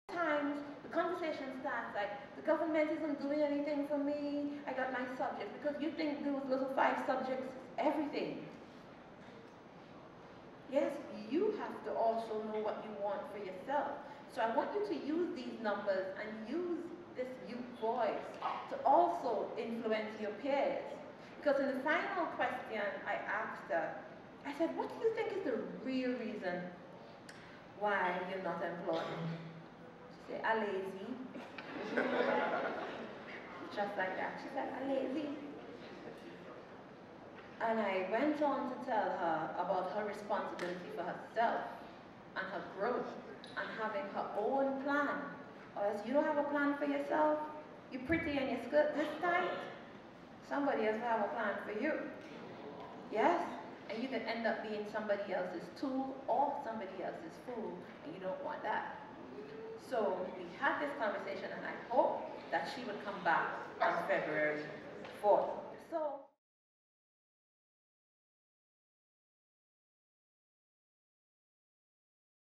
IN HER WORDS: Shamfa Cudjoe
Trinidad’s Minister of Sport and Youth Affairs Shamfa Cudjoe made this point on last week in response to youth leaders who voiced concerns about not having decision-making power in matters that affect them. She was speaking during the opening ceremony of the 6th Caribbean Youth Leaders Summit at the Hyatt Regency, Port of Spain. (Video by Tre Greaves)